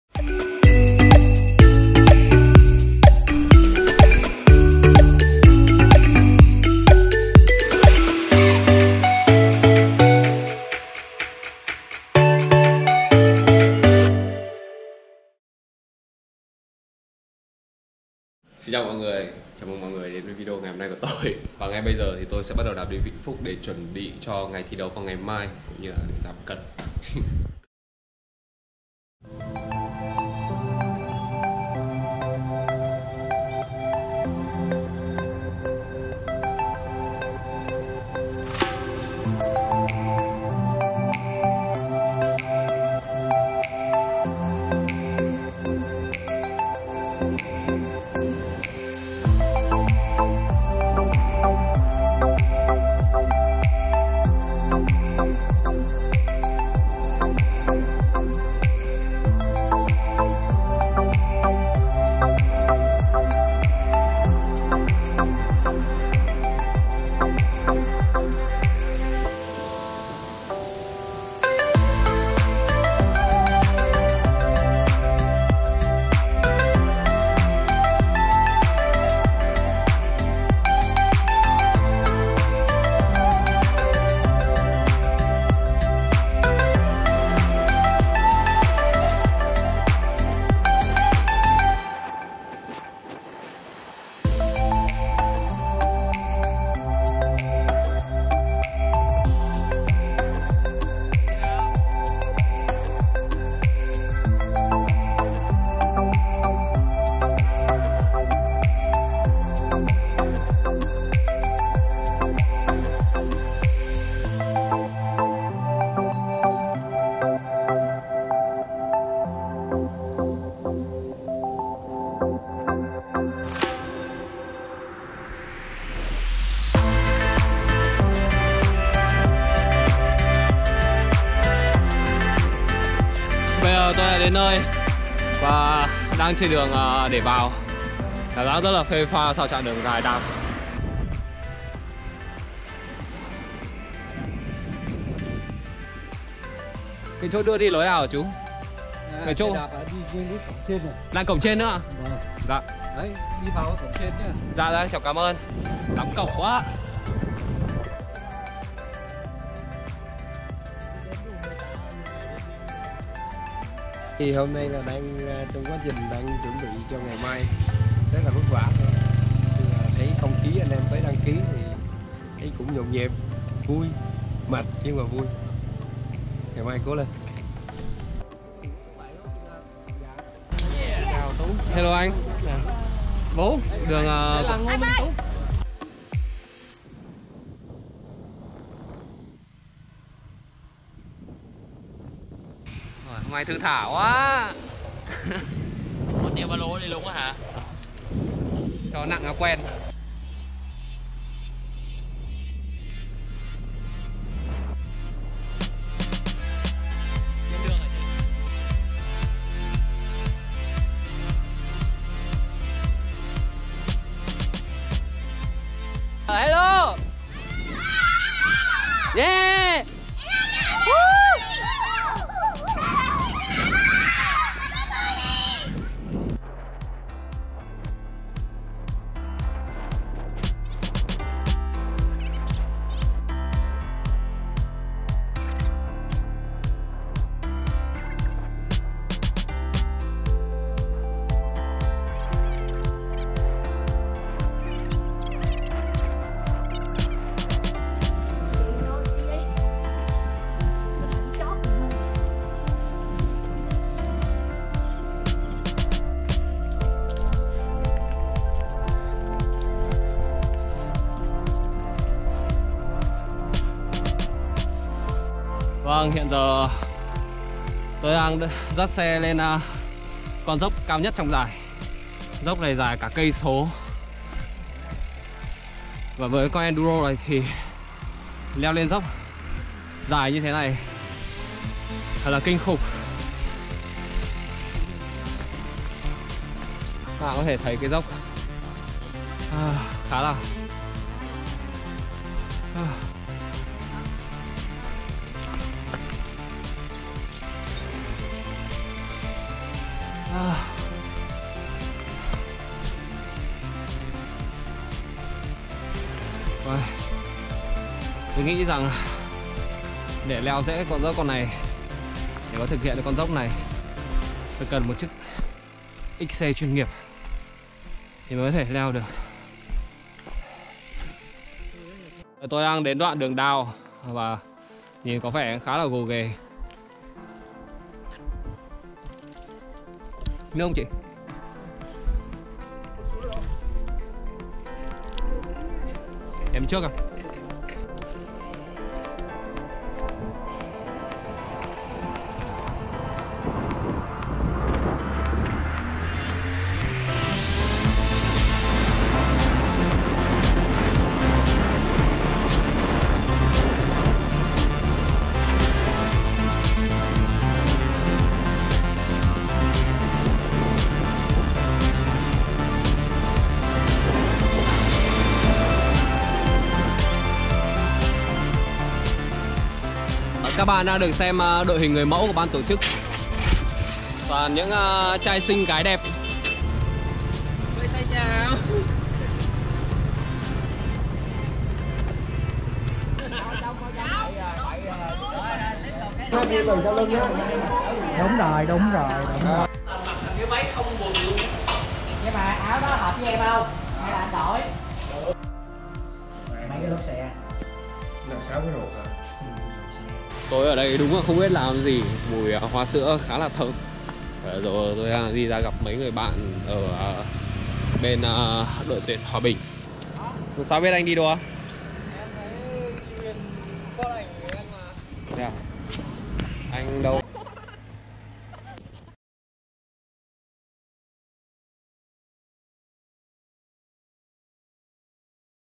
Thử Thách Đạp Xe Từ Hà Nội Tới Vĩnh Phúc Để Tham Gia Giải Đua Xe Đạp Địa Hình | Ngô Minh Tú Vlog #28
Hãy subscirbe để tiếp tục theo dõi những videos của mình nhé mọi người ;)